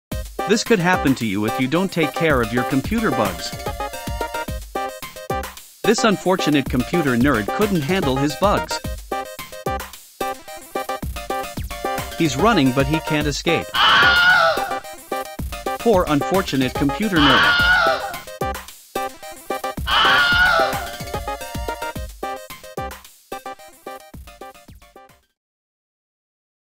Computer nerd Attacked by Giant Computer Bugs
Computer Nerd chased by giant computer bugs he was working to remove, but failed! Video created using images from Midjourney AI and animated by Pika Labs. More to come!

By the way, I created the music using Garageband.